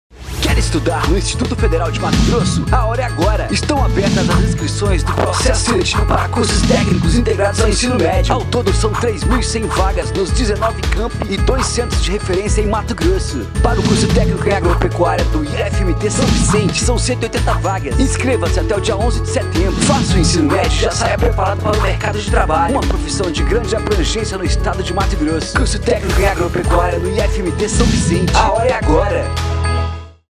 Processo Seletivo IFMT 2023-01- Técnico em Agropecuária
O EDITAL SAIU PRA VOCÊ ENTRAR 🏃‍♀️🏃‍♂️🏃_

💚 Você tem interesse em estudar no Instituto Federal de Mato Grosso? A hora é agora!

⏳ As inscrições do Processo Seletivo 2023/1 para os Cursos Técnicos Integrados ao Ensino Médio estão abertas até 11 de setembro de 2022.
Para o IFMT São Vicente são 180 vagas no curso TÉCNICO EM AGROPECUÁRIA.

💰A taxa de inscrição é de R$ 50,00 (cinquenta reais).

🙋‍♂️🙋‍♀️ A isenção da taxa de inscrição pode ser solicitada por candidatos(as) que se autodeclararem de baixa renda e comprovarem sua condição, encaminhando os documentos exigidos até 20 de julho de 2022.

Ao todo são 3.100 vagas nos 19 campi e 2 centros de referência em Mato Grosso. 🏫

📚 A prova objetiva será no dia 09/10/2022.